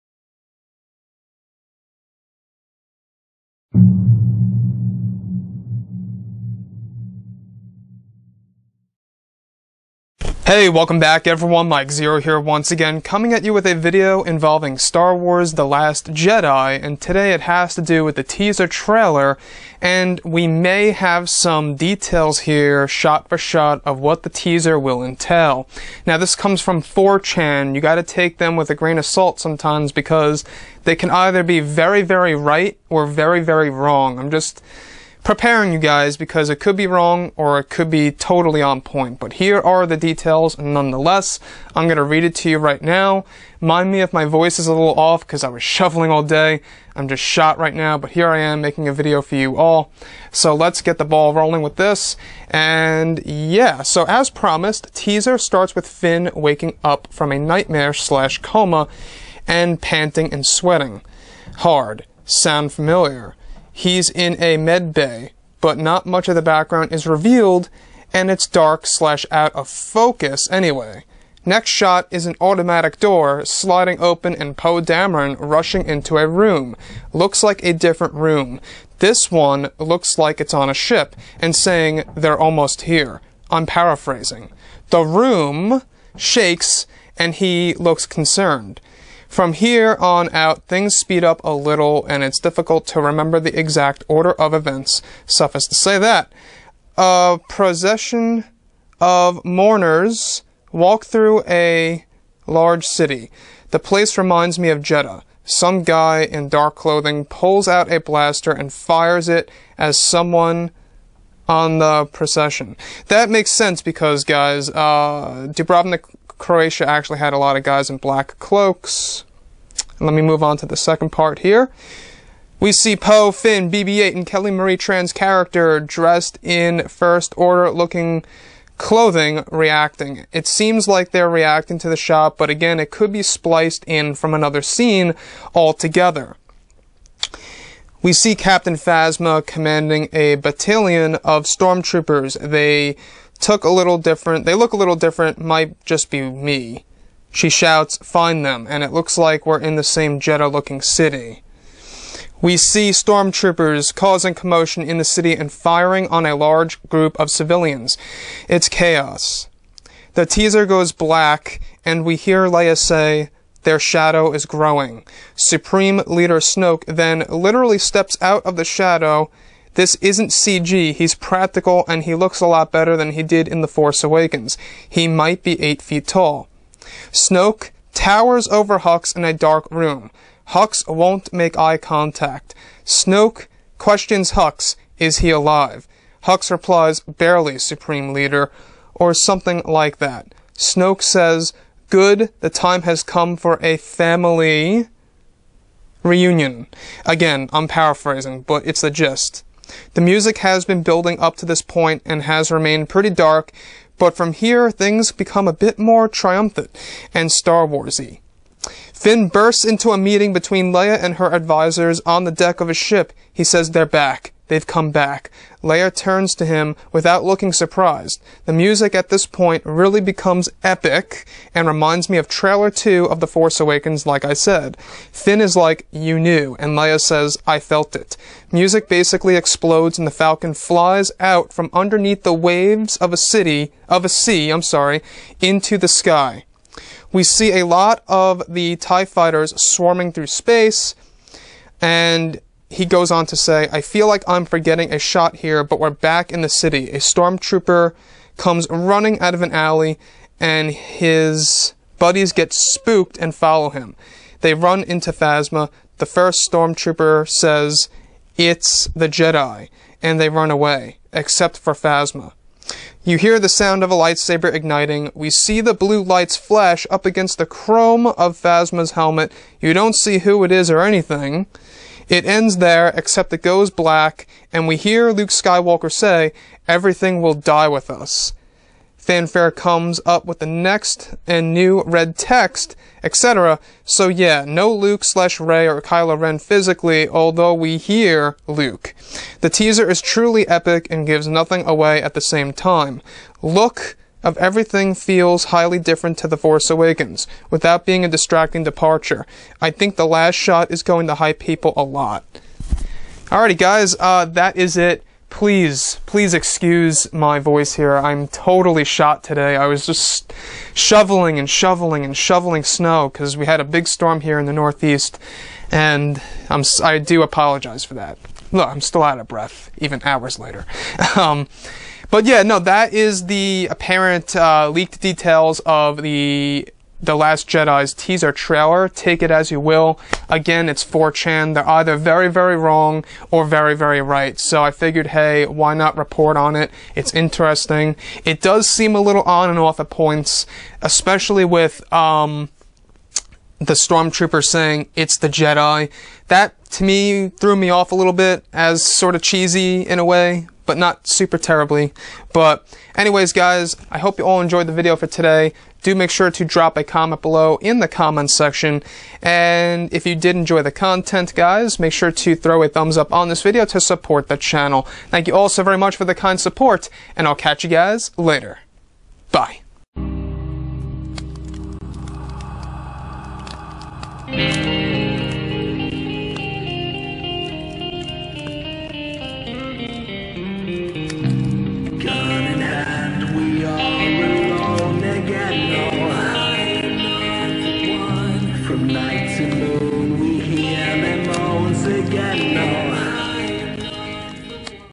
Star Wars Episode 8 The Last Jedi Teaser Trailer Details Leaked
Today we go over a Star Wars Episode 8 The Last Jedi Teaser Trailer leaked details by 4Chan. This Star Wars Episode 8 trailer information is interesting but do keep in mind 4chan can be right and or wrong many times.

WATCH MY LAST VIDEO (STAR WARS EPISODE 8 THE LAST JEDI KYLO REN DARTH VADER HELMET VISION)